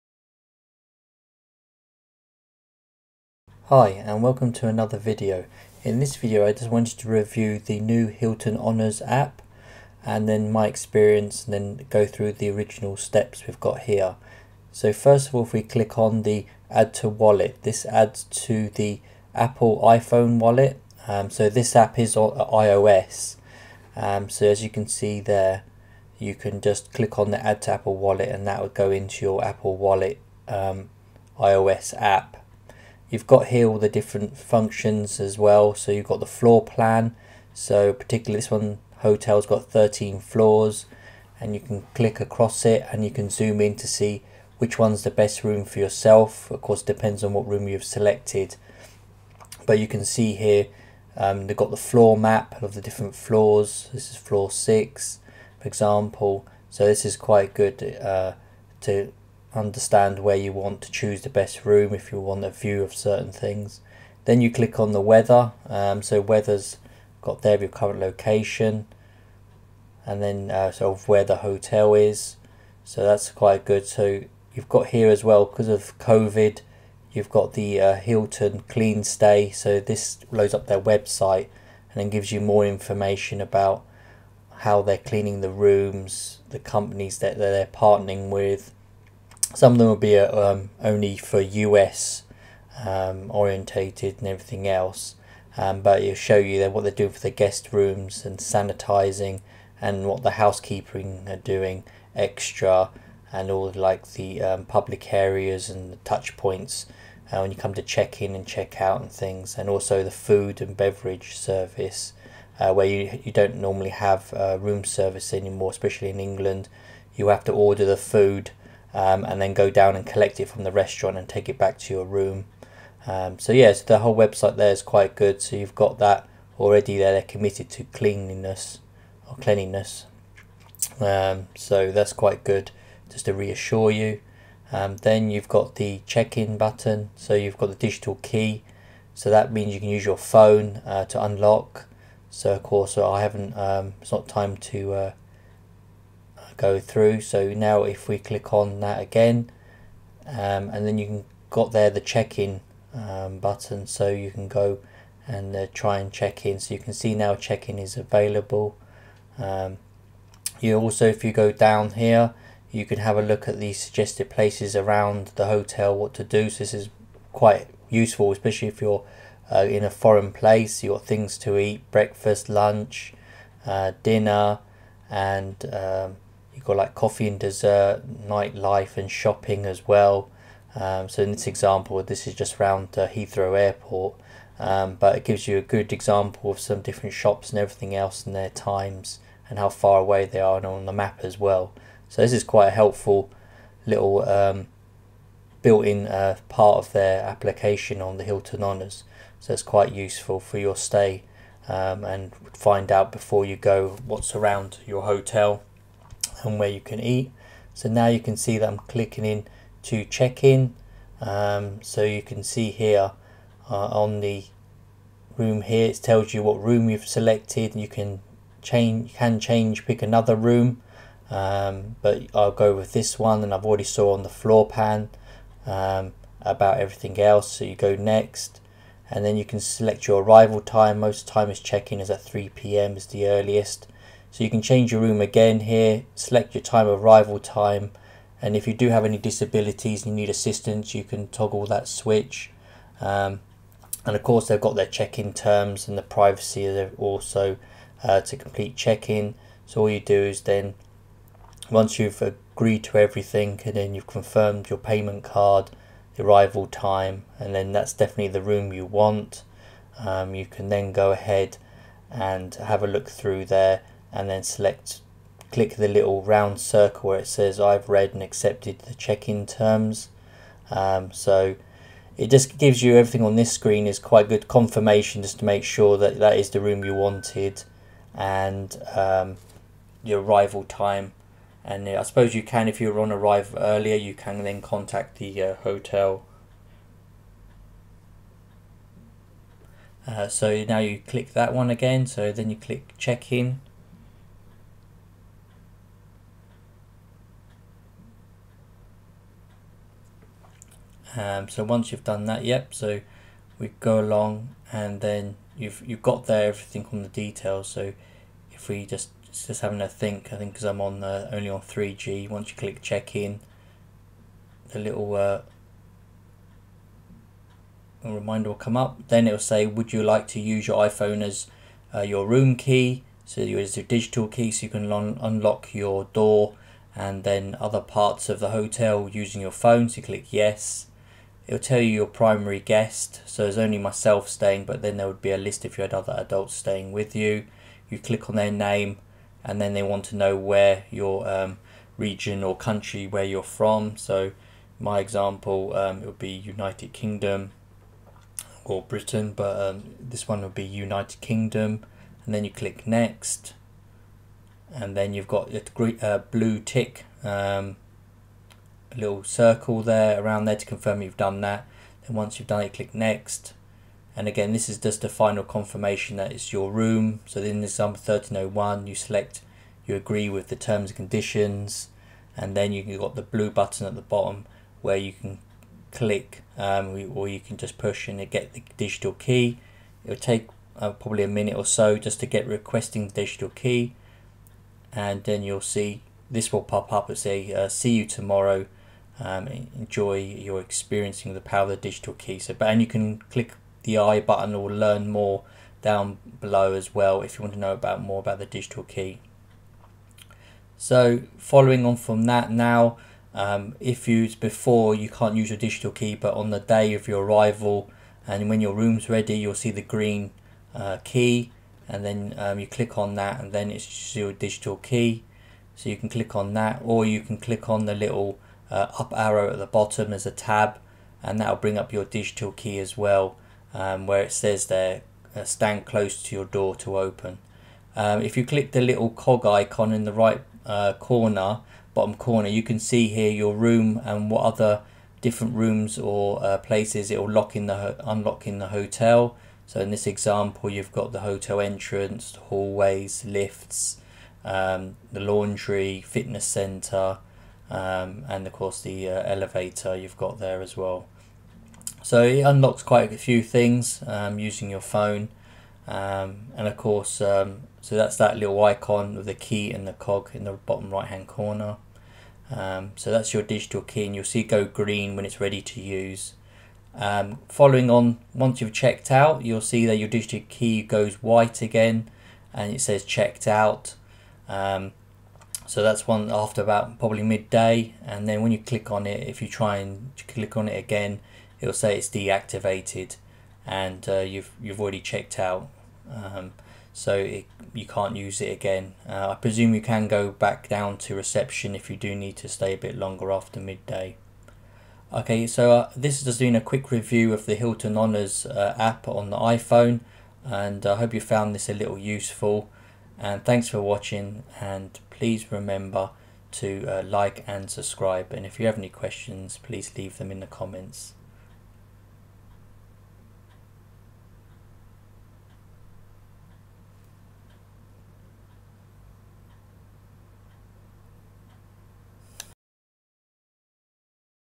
Hilton Honors App 2021 Review - Digital Key, Check-In/Out, Member Benefits, & App Features
A quick review of the Hilton Honors App, showing how to prepare/use the Digital Key and Key Features within the App. The app allows you to see your Gold, Diamond member benefits.